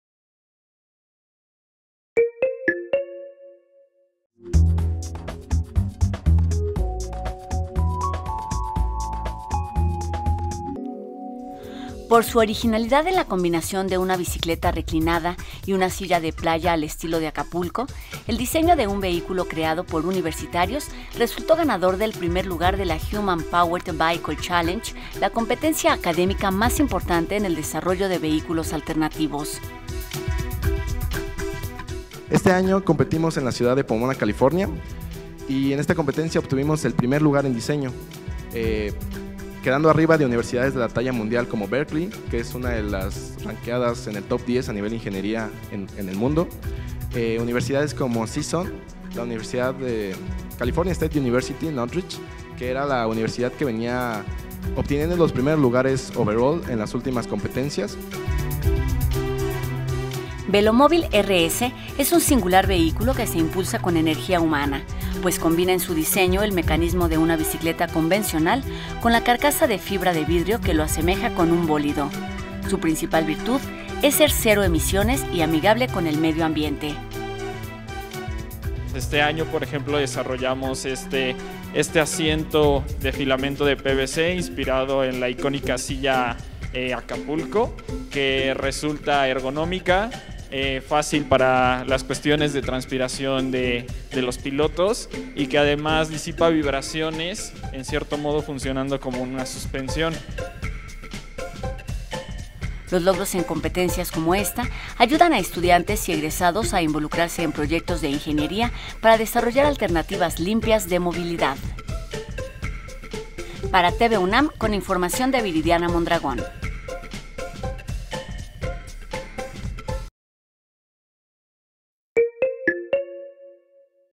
VELOMÓVIL RS FACULTAD DE INGENIERÍA
“Velomóvil RS” es propulsado por energía humana, combina el funcionamiento de una bicicleta reclinada con una carcasa de fibra de vidrio.
Con este desarrollo, un equipo de estudiantes y egresados de la Facultad de Ingeniería ganaron en el rubro de diseño en el Human Powered Vehicle Challenge, en Estados Unidos
Diseño, innovación, velocidad y resistencia, criterios de evaluación
Este singular vehículo estudiantil combina el mecanismo de una bicicleta muy reclinada, un asiento inspirado en las sillas playeras de Acapulco, compuestas por alineadas cuerdas plastificadas, y una carcasa de fibra de vidrio que se divide en dos partes para acercarse al diseño de un bólido.